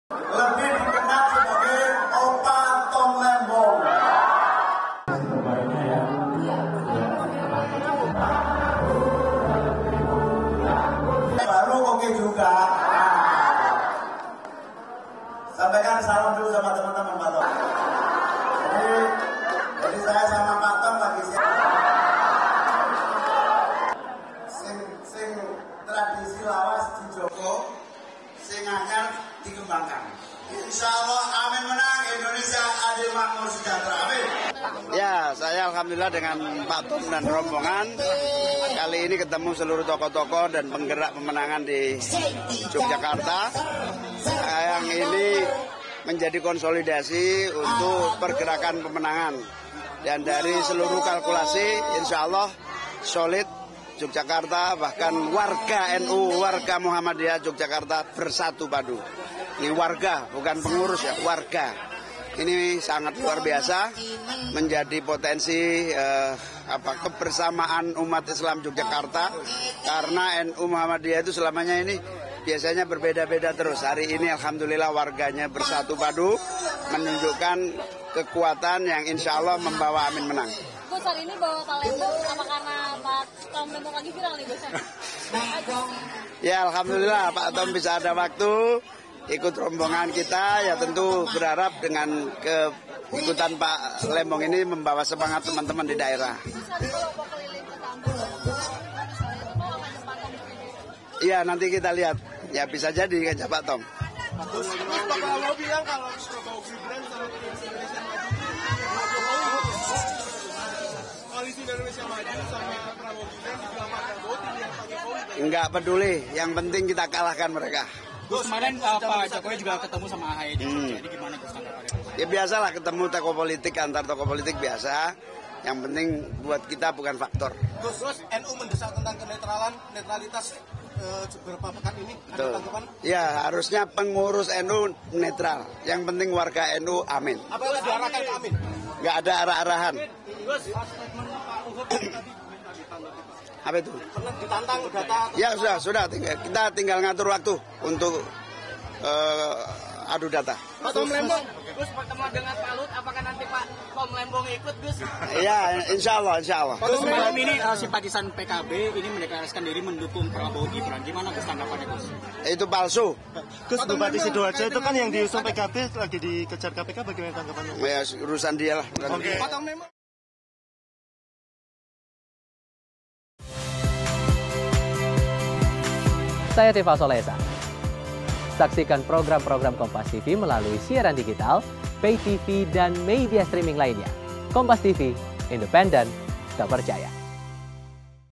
Begini Momen Cak Imin Didampingi Tom Lembong saat Kampanye di Yogyakarta
YOGYAKARTA, KOMPASTV  Cawapres nomor urut 1, Muhaimin Iskandar kompak bersama Tom Lembong di depan rapat umum rakyat Yogyakarta di Pura Seni, Senin (29/1/2024).

"Mengambil baru yang baik, yang lebih baik, yang akan datang, namanya Pak Tom Lembong. Lebih dikenal lagi sebagai Opa Tom Lembong," kata Cak Imin dalam sambutannya di Pura Seni Yogyakarta, Senin (29/1/2024).

Cak Imin pun blak-blakan dirinya dan Tom tengah bersiap menghadapi 'Opung'.

"Jadi saya sama Pak Tom lagi siap-siap ngadepi Opung," ungkap Cak Imin.